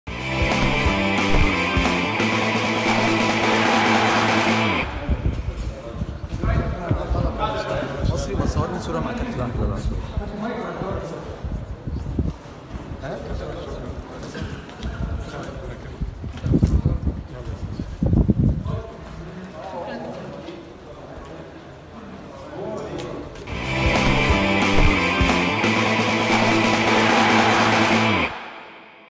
لحظة وصول محمود الخطيب لمتابعة مران الأهلي الأخير لمواجهة الترجي
تقرير:  أحمد عبدالباسط
مونتاج : اسلام فرغلي